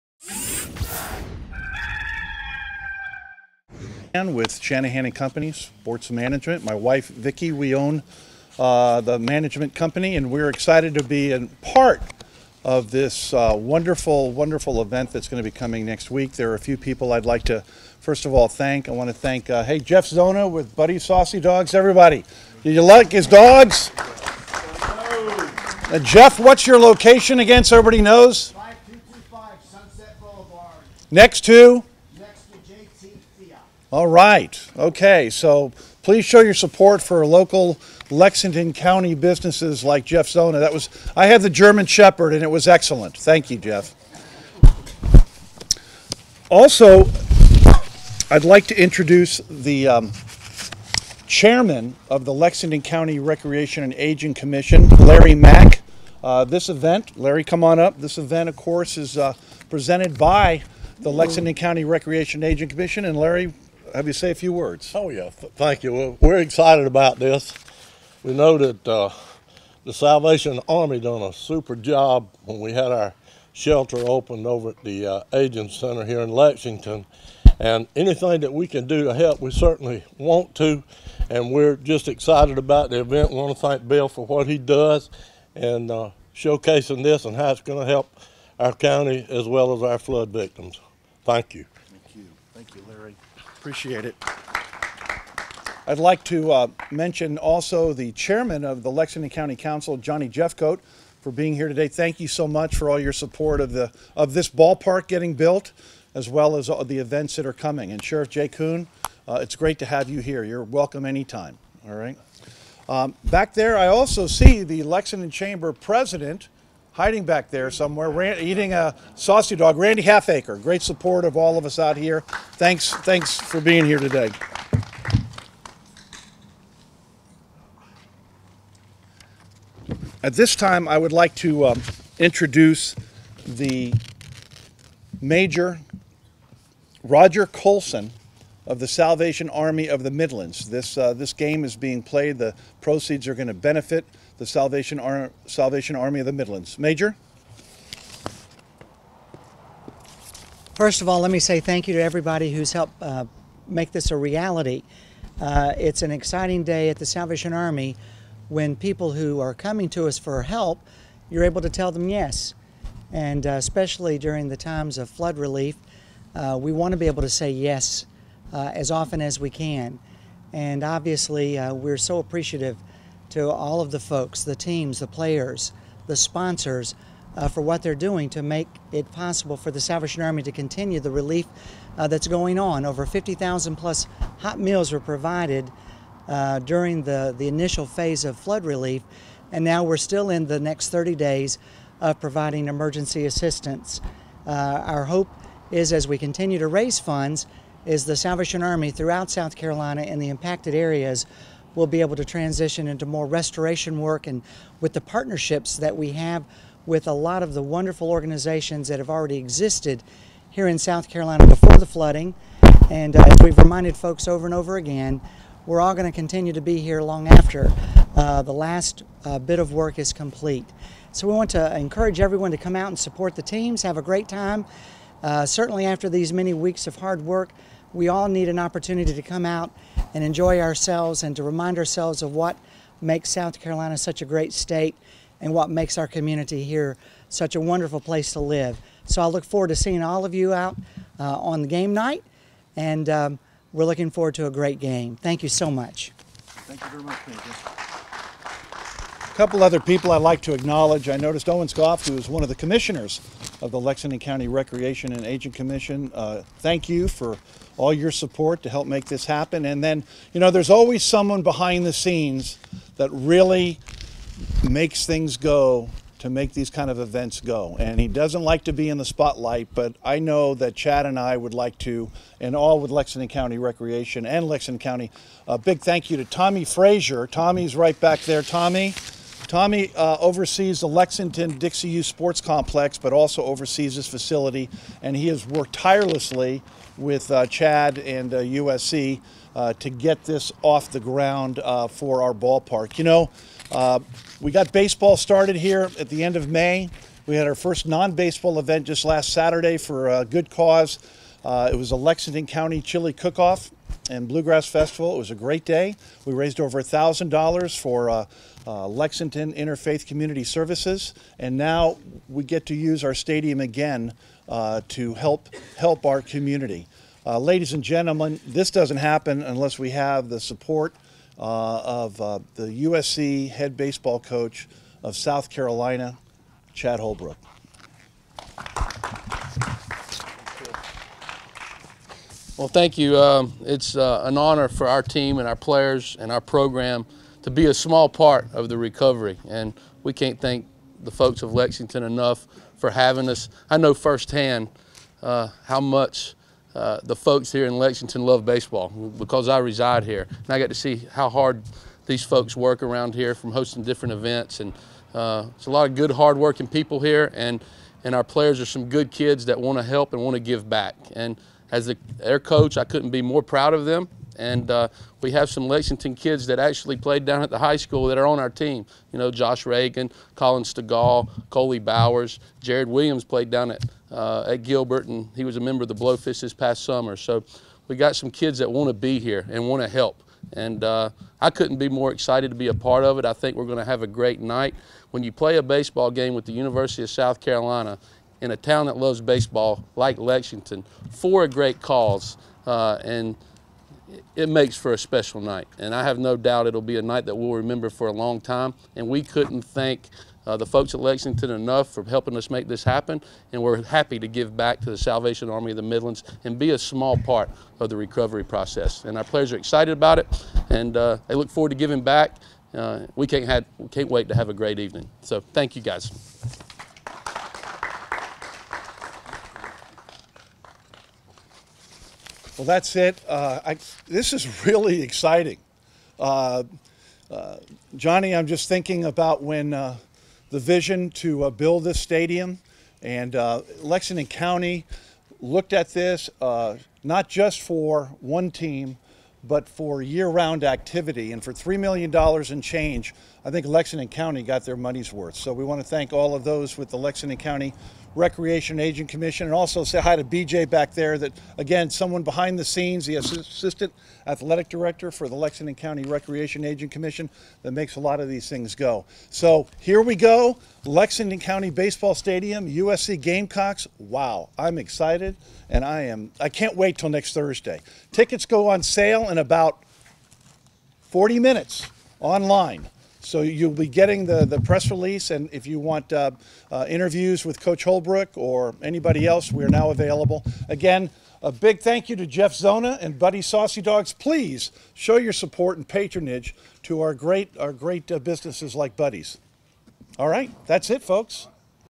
Garnet & Black World Series Announcement - 10/30/15
Official announcement that the Gamecocks will play a Garnet & Black World Series game at Lexington County Baseball Stadium on Thursday, Nov. 5, at 7 p.m.